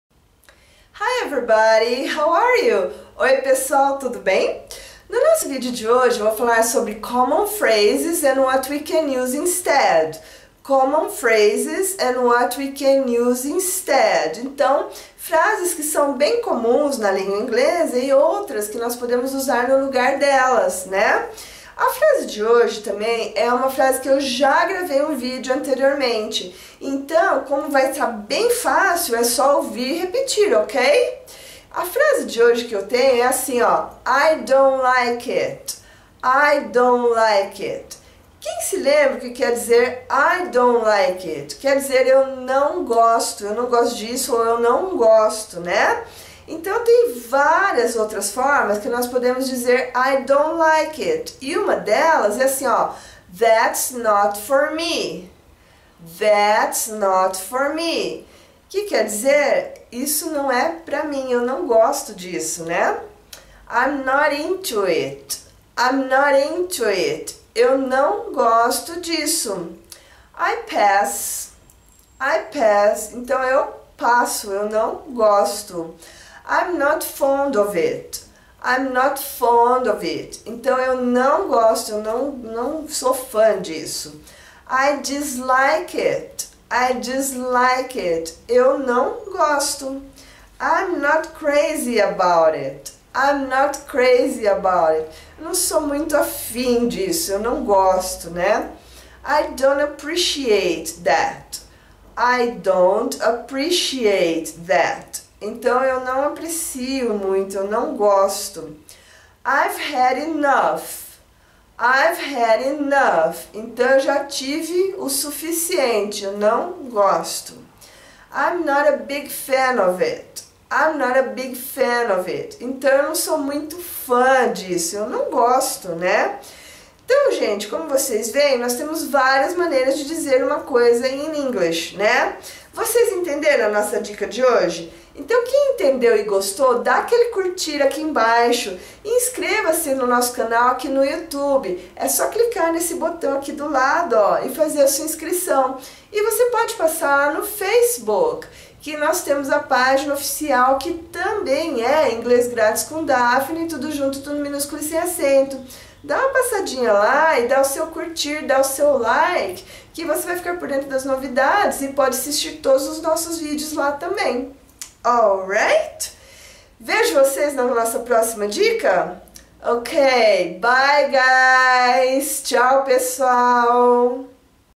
não gosto - fale inglês rápido no YouTube - inglesgratiscomdaphny
Neste vídeo vamos rever algumas maneiras de se expressar de forma natural em inglês usando a frase EU NÃO GOSTO, através de exemplos e figuras fáceis de entender. Vamos aprender inglesgratiscomdaphny aqui no YouTube?? E depois de assisitir, é só fazer sua inscrição no canal para acessar mais vídeos gratuitos. Bons estudos, bjssss